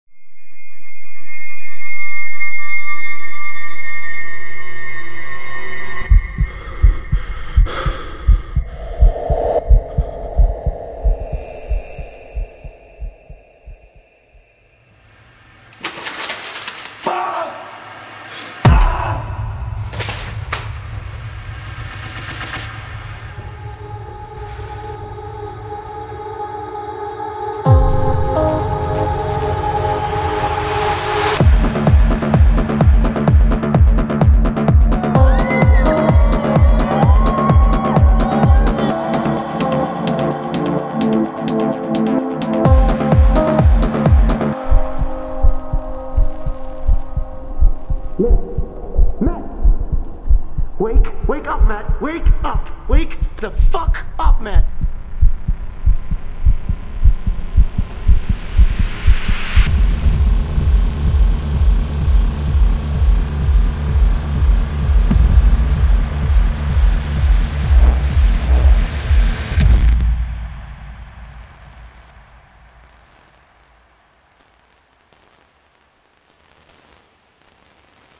Digital Cortex
The Digital Cortex. A trailer for my on going passion project which is in development to become a short movie.

Behind the scene's and more?